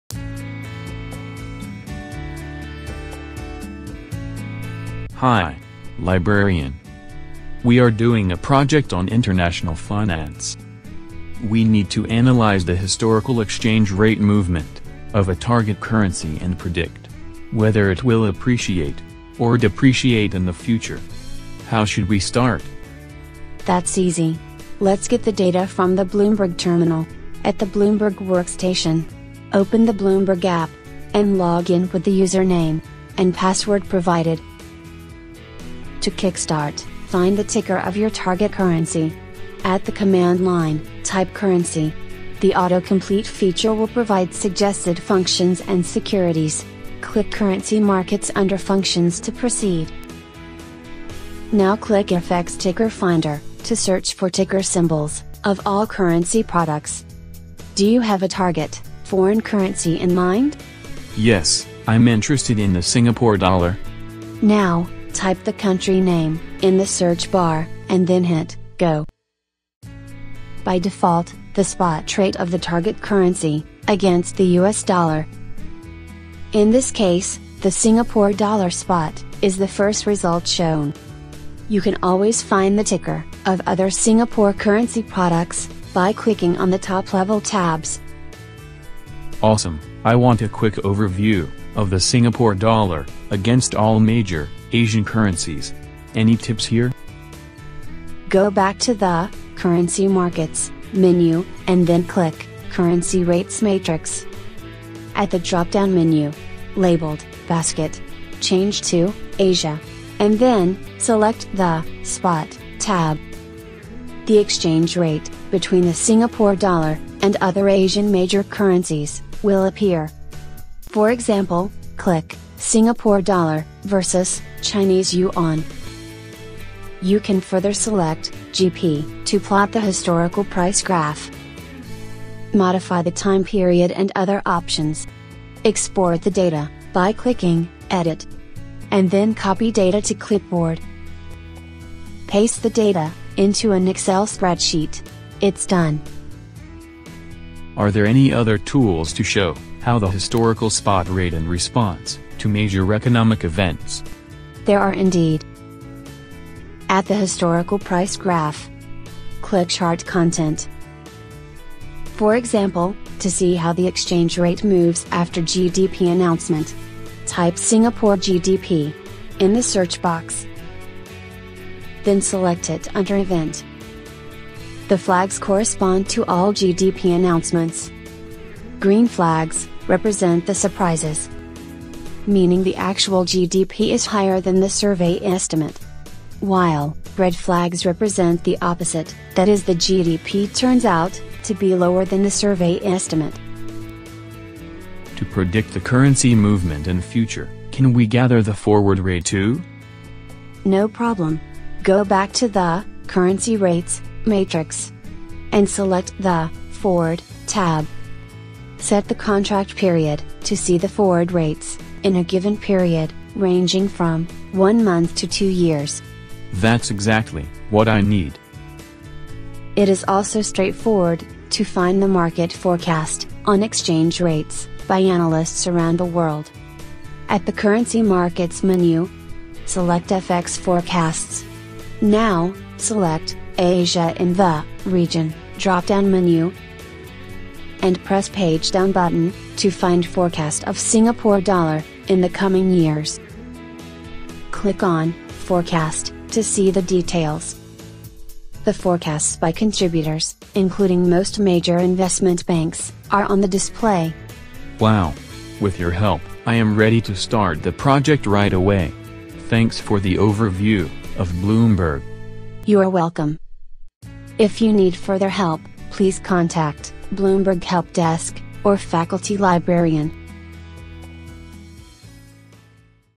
Finding Spot Rate, Forward Rate and Market Forecast of Currency
This Bloomberg video is prepared by Dr Anson Wong (AF), Dr Derek Yim (AF), and Mr William Ho (LIB) from the Hong Kong Polytechnic University, with funding support from UGC Teaching & Learning Project on enhancing information literacy in Hong Kong higher education through the development and implementation of shared interactive multimedia courseware (IL Project) from year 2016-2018.